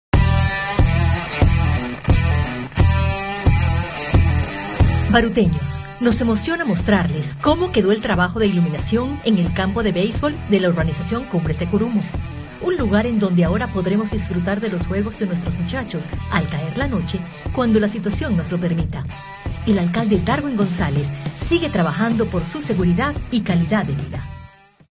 ¡Iluminamos el Polideportivo de Cumbres de Curumo!
En estos días estuvimos cumpliendo con otra de nuestras obras solicitadas en nuestro presupuesto participativo 2021: la recuperación del Polideportivo de Cumbres de Curumo.

Instalamos luces LED en todo este espacio, así todos los vecinos podrán hacer deporte y disfrutar de los juegos de nuestros jóvenes y niños en horas de la noche y de forma segura, cuando la situación lo permita.¡Seguiremos cumpliendo nuestros compromisos y recuperando los espacios deportivos!